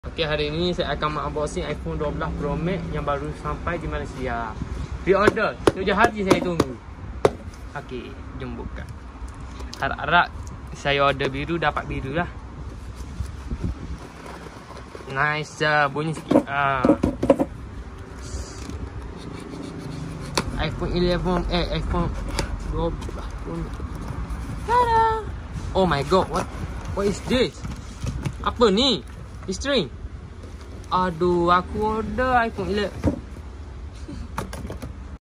Iphone 12 pro max 64GB GOLD unboxing
Iphone 12 pro max unboxing,hari yang ditunggu2,akhirnya sampai jgk